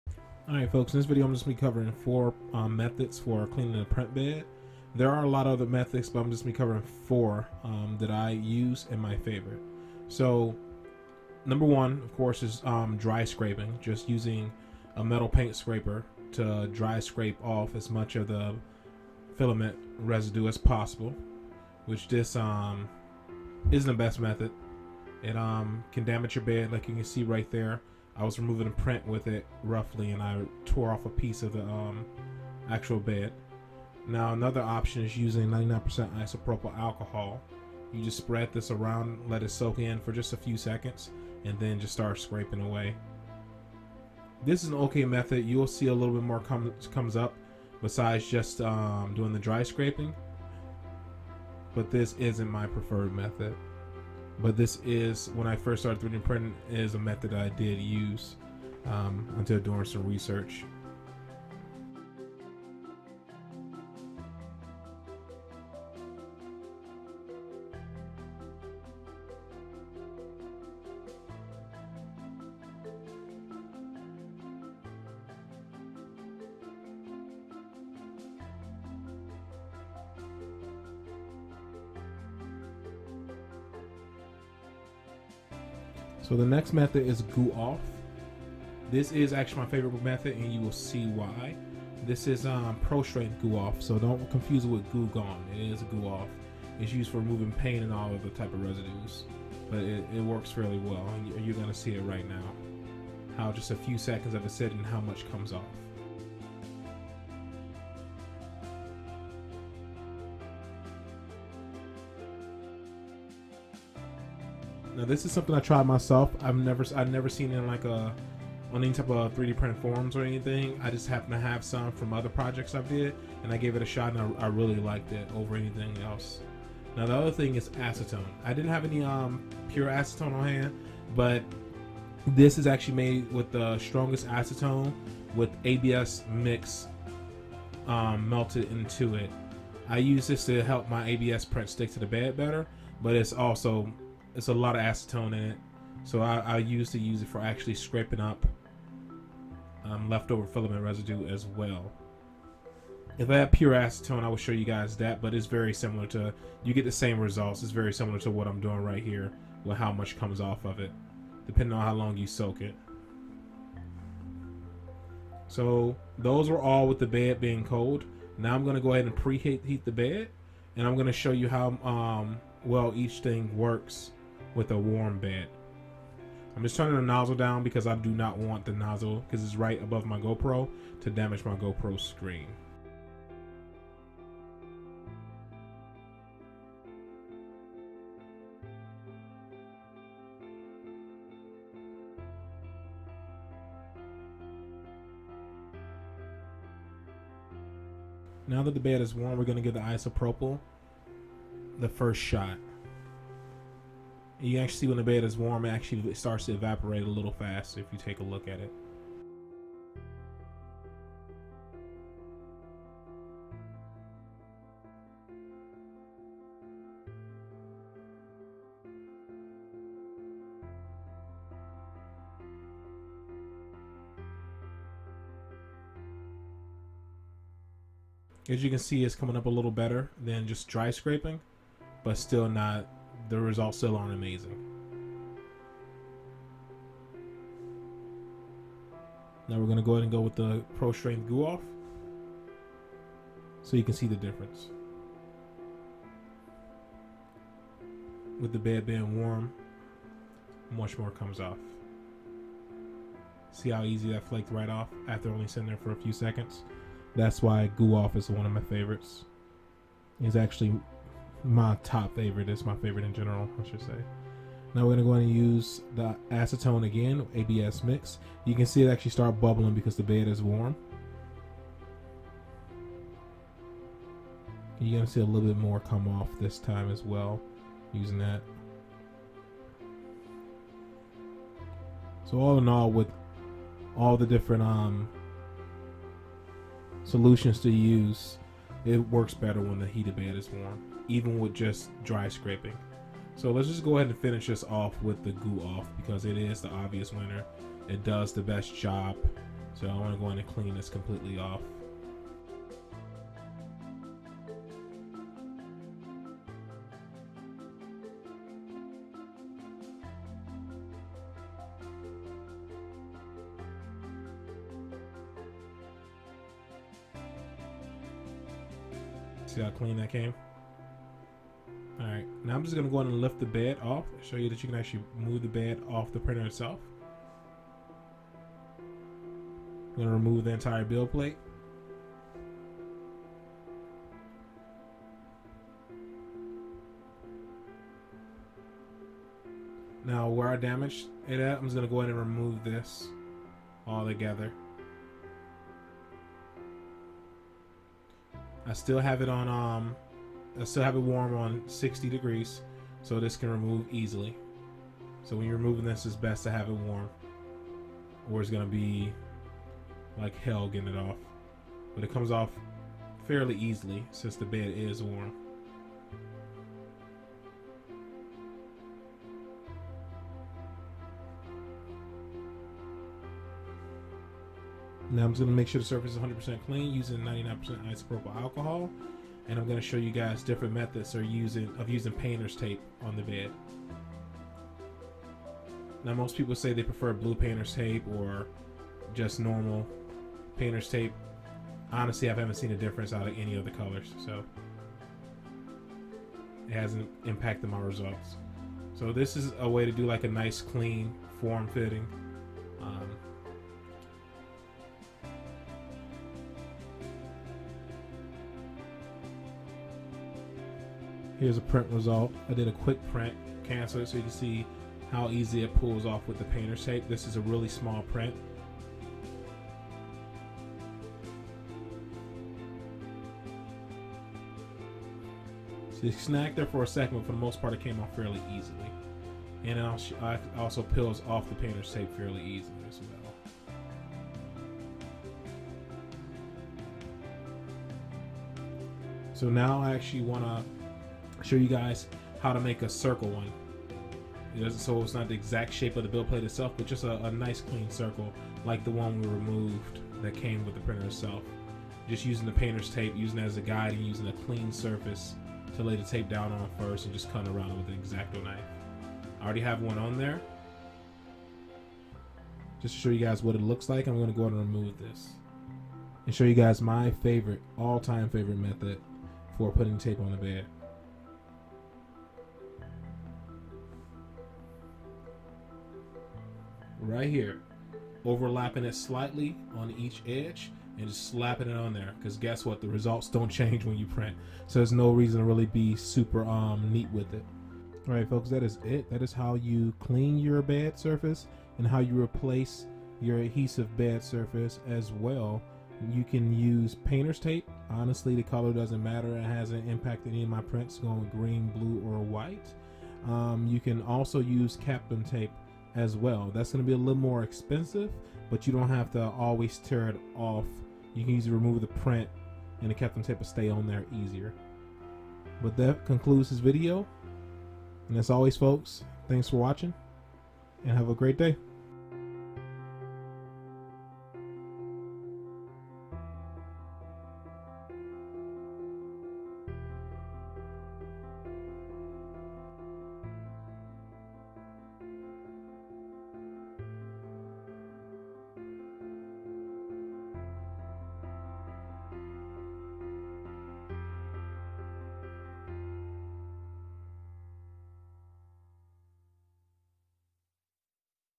Mono Price Delta Cleaning , Replacing Build Surface and Removing Prints
Don't Click this Mono Price Delta Cleaning, Replacing Build Surface and Removing Prints